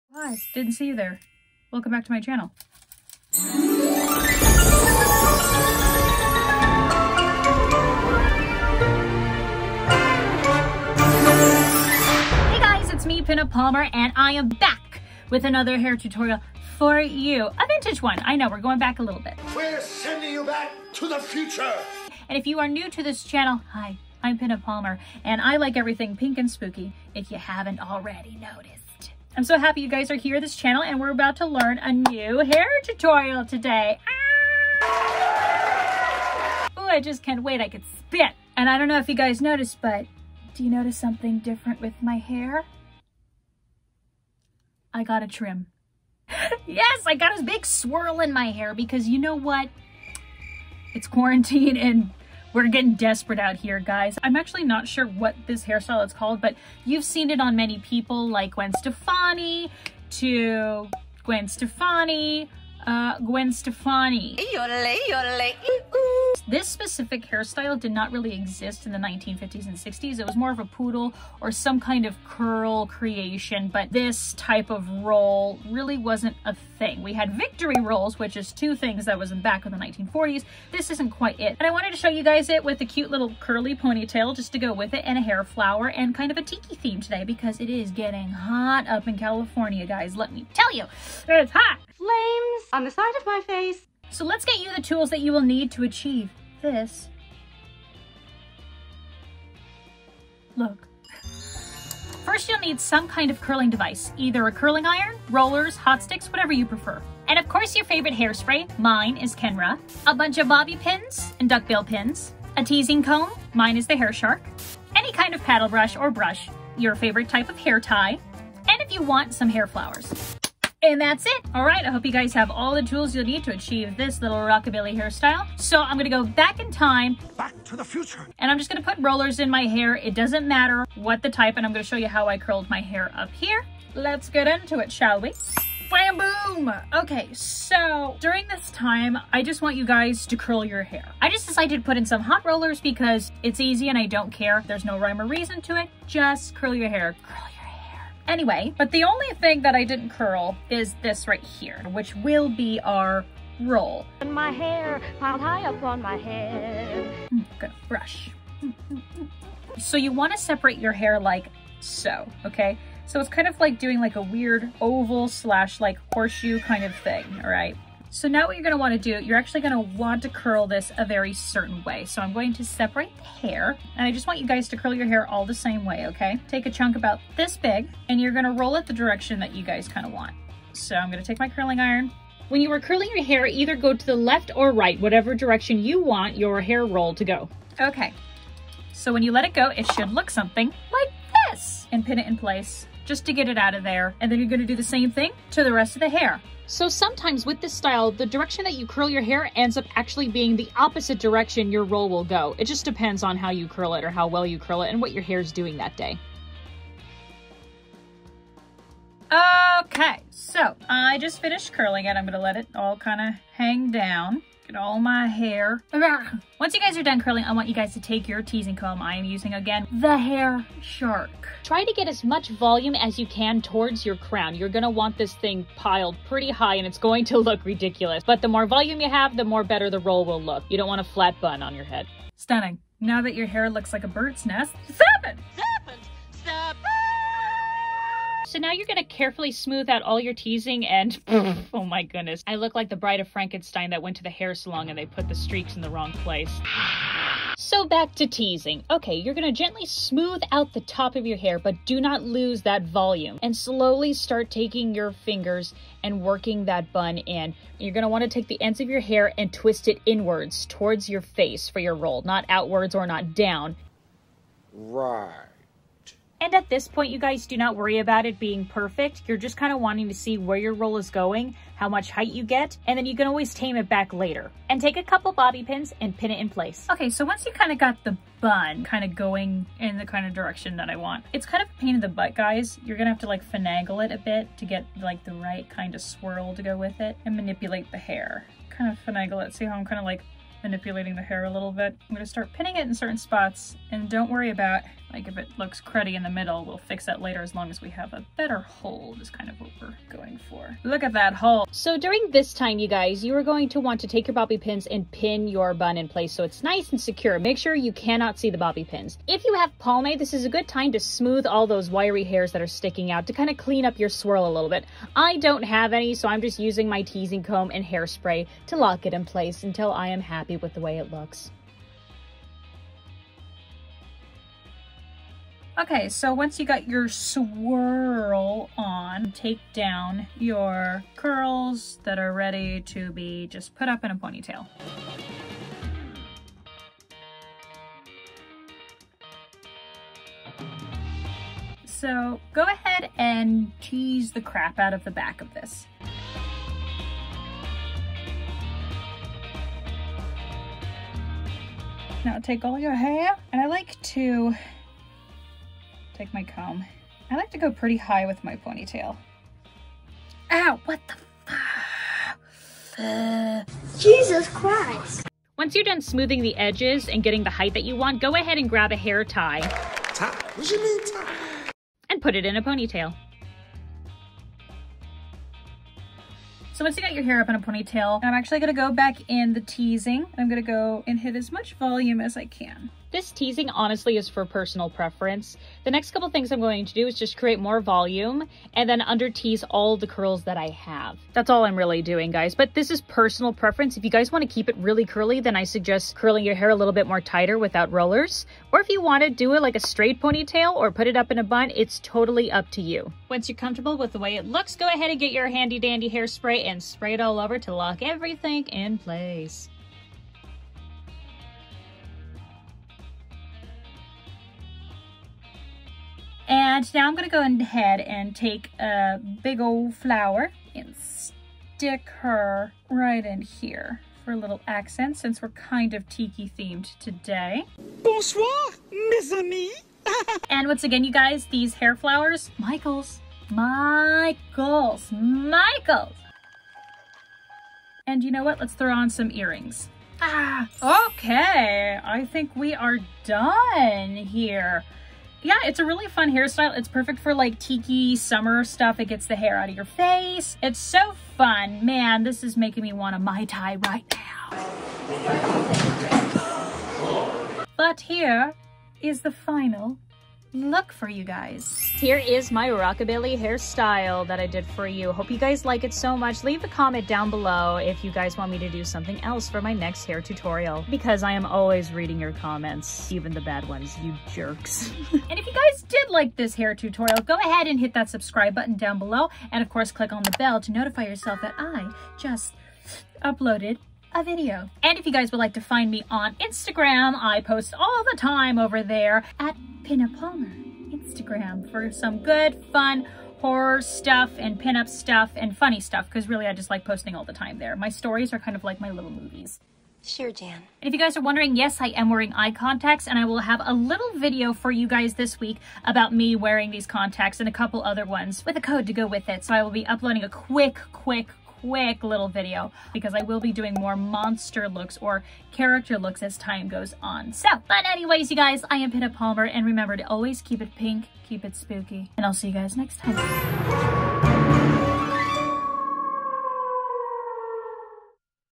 Pinup Ponytail Tutorial
Hey Pinups! Here is a quick tutorial on a iconic rockabilly hairstyle! I also paired it with a simple ponytail! Just in time for summer! This iconic hairstyle will turn you into a instant pinup girl!

Art by - Unfortunate_ boy
Halloween sub pack by Esthergt310
Green Screen By - IEditingX
 Zombie tiki dress - violetinmay
Earring -  vintage